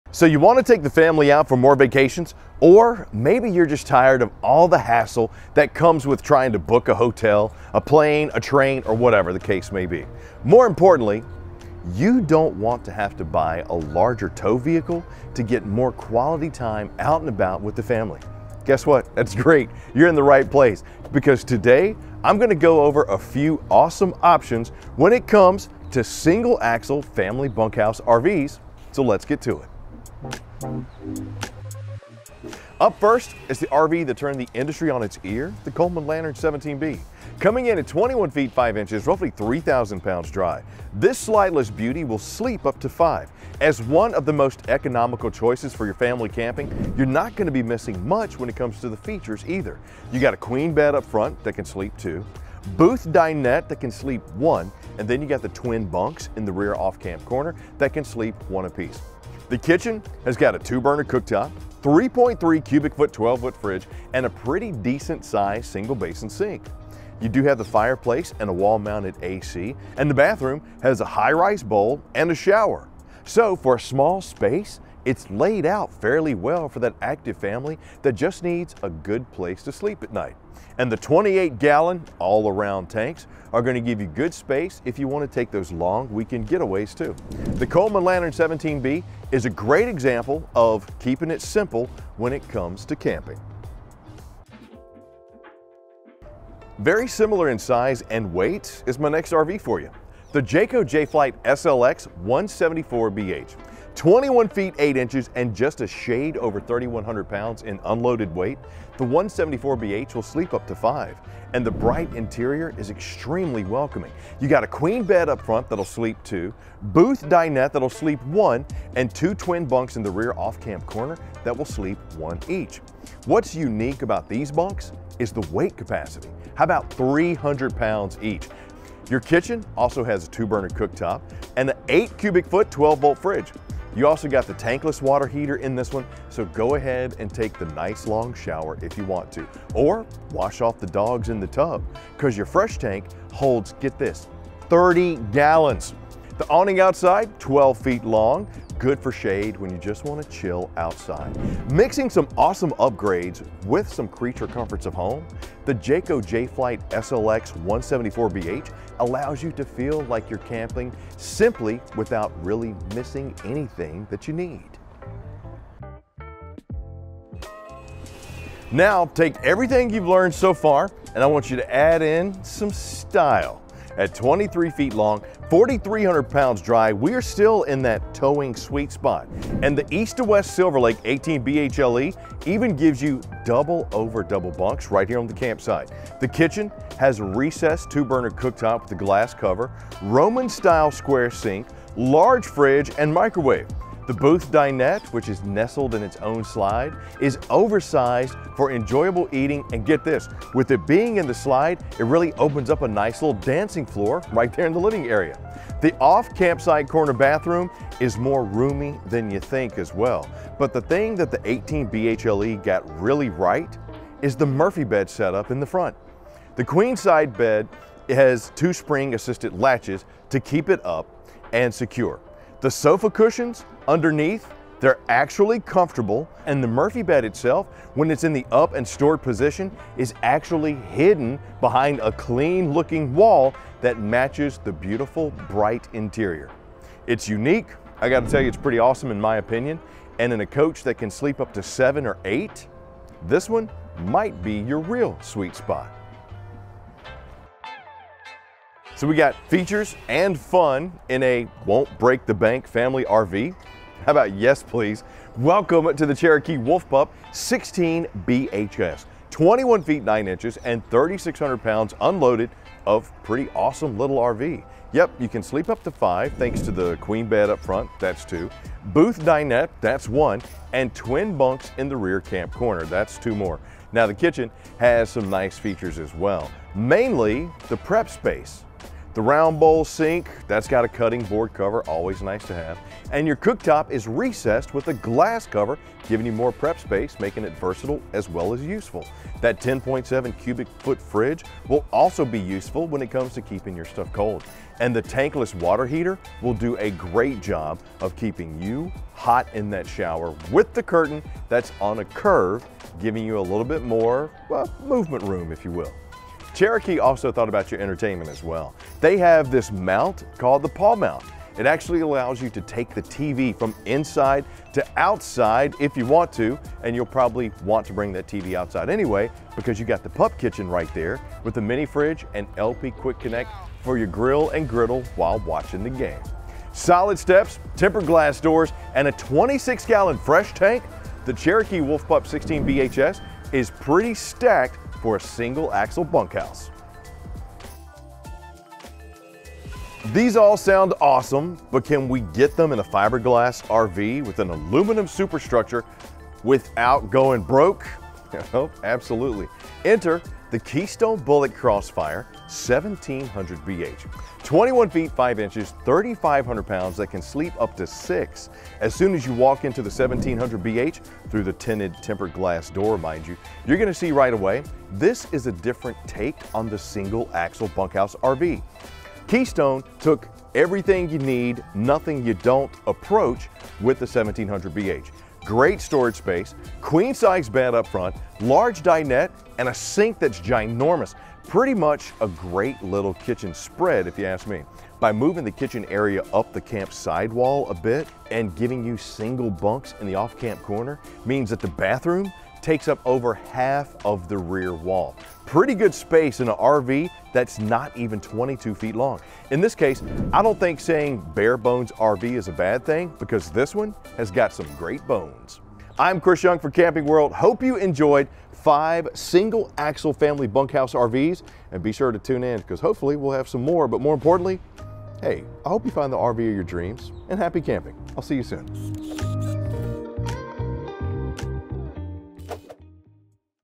Lightweight & Affordable Options | RV Buying Guide
A single-axle travel trailer is perfect for families with an SUV or smaller truck, but it can be difficult to find a camper that fits the whole family. So we’ve made this list of the top five small campers with bunkhouses that can fit the kids without you needing to buy a new tow vehicle. Check out this collection from Coleman, Jayco, East to West, Forest River, and Keystone.

Take a look at all the design choices that allow for additional sleeping spaces, even in a small travel trailer, including double-over-double bunks, convertible dinettes, hidden Murphy beds, and more. And that’s just solving for sleeping spots. These single-axle travel trailers don’t sacrifice features, and you’ll find spacious showers, outdoor TV capability, tankless water heaters, large refrigerators, outdoor kitchens, and more.